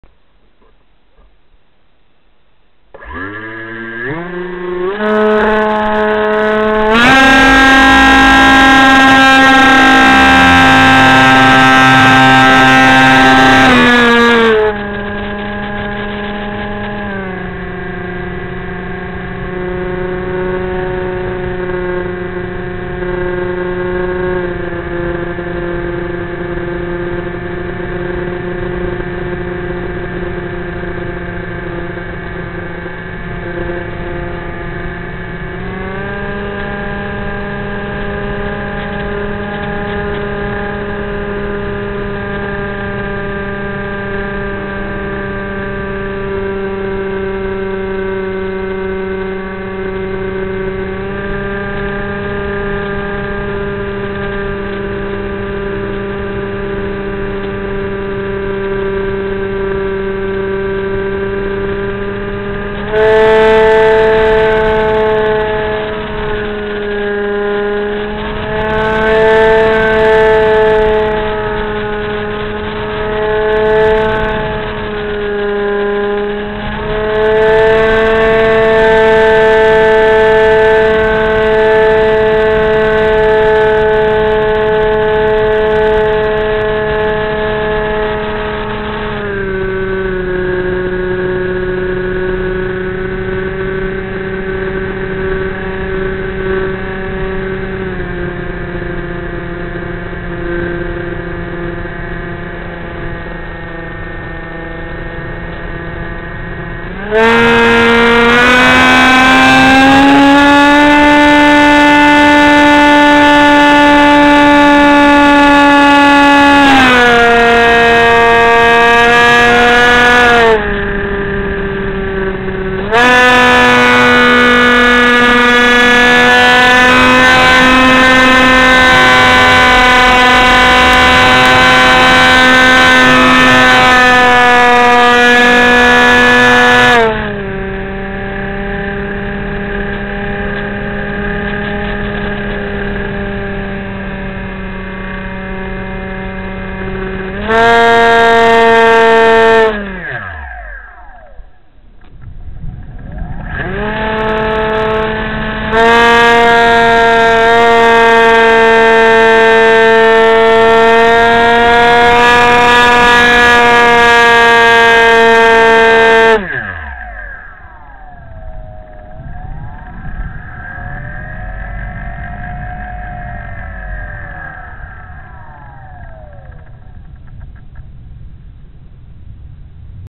Teksumo: First flight Oct 2016: 720p
Quick flight with my new teksumo. Using 2200 25c 3 s lipo, 7x6 prop 50amp hobbywing esc, 2 SG90 servos. Now upgraded with MG90s, 1400 kv motor and 8x6 prop. Nice flying wing, great fast or slow, cheap from hobbyking.co.uk  but mine seems to 'wobble' in flight. Currently looking at correx to make larger wing tips...